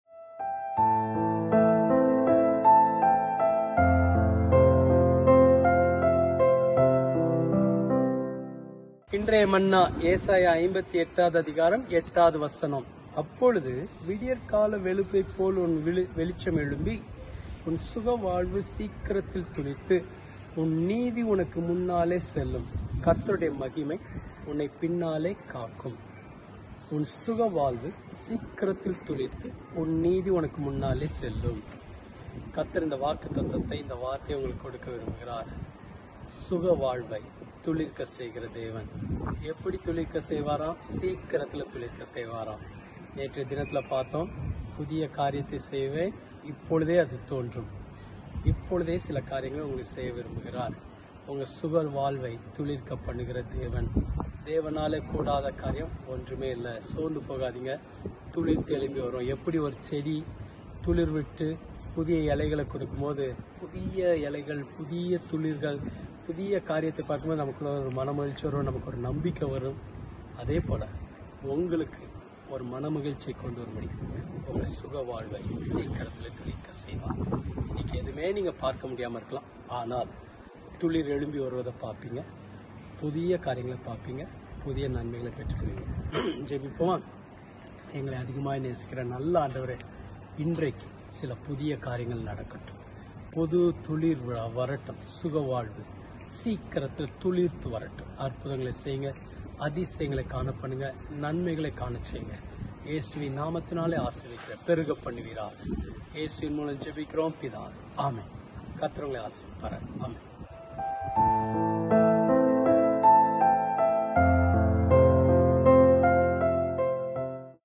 உன் சுகவாழ்வு சீக்கிரத்தில் துளிர்த்து (ஏசாயா‬ ‭58‬:‭8‬) - Thought for the Day(Tamil) - 03Oct2024
“அப்பொழுது விடியற்கால வெளுப்பைப்போல உன் வெளிச்சம் எழும்பி, உன் சுகவாழ்வு சீக்கிரத்தில் துளிர்த்து, உன் நீதி உனக்கு முன்னாலே செல்லும்; கர்த்தருடைய மகிமை உன்னைப் பின்னாலே காக்கும்.” (ஏசாயா‬ ‭58‬:‭8‬)

Thought for the Day(Tamil) - 03Oct2024